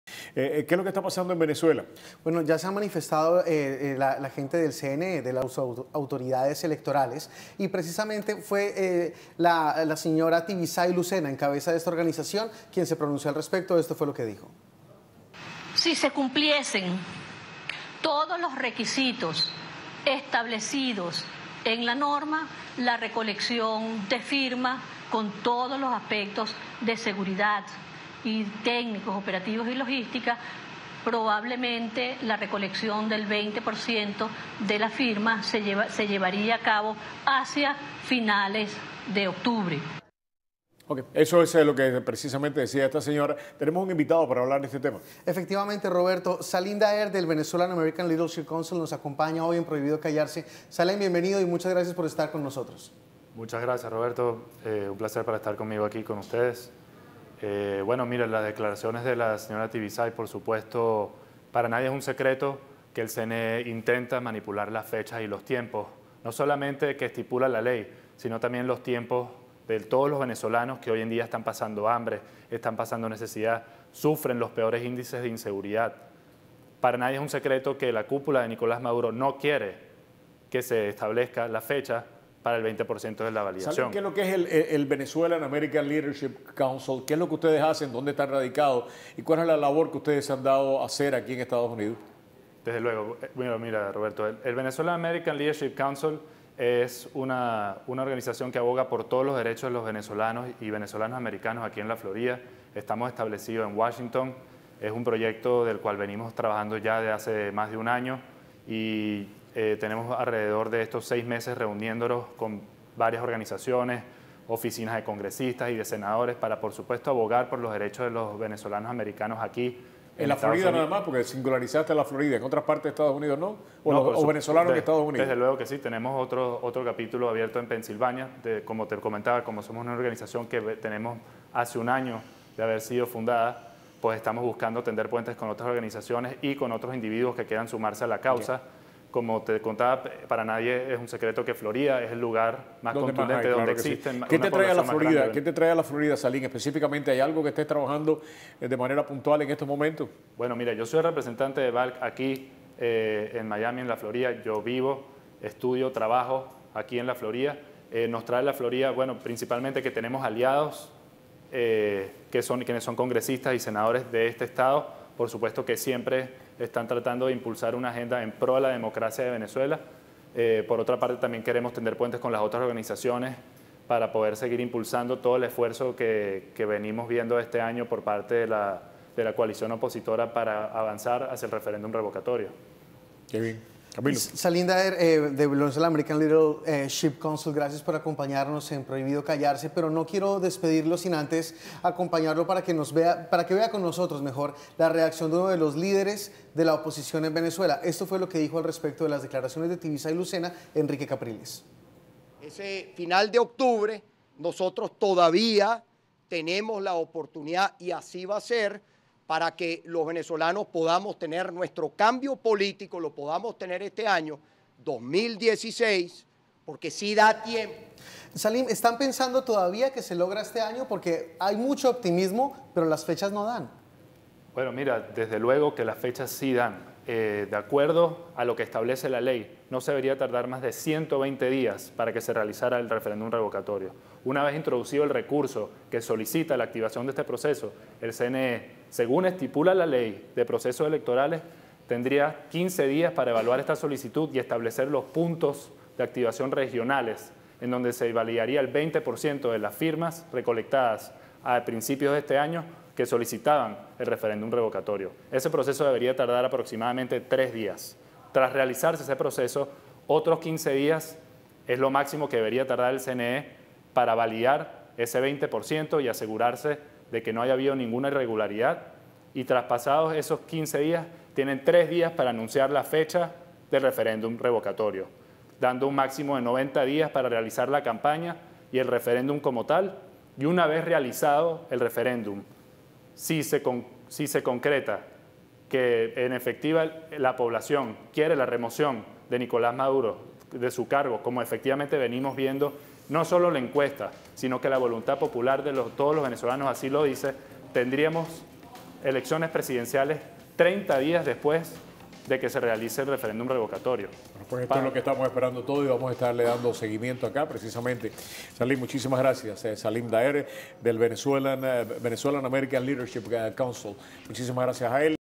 CNE pide 20%, del Padrón Electoral
Juan Camilo Gómez, Periodista y Salim Daher, Venezuelan American Leadership Council, conversa con Roberto Rodriguez Tejera sobre el Referendum Revocatorio en Venezuela.